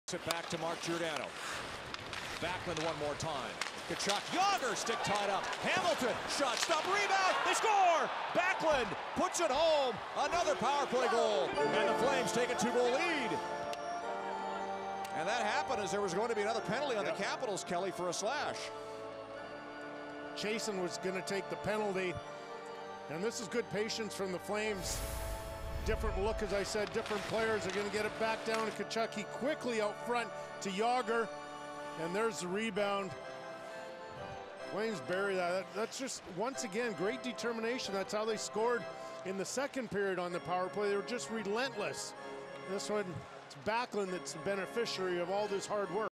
Mikael Backlund Goal vs WSH 11-20-17
Mikael Backlund 5 goal of 2017-18 nhl season. pp goal assisted by Dougie Hamilton and Matthew Tkachuk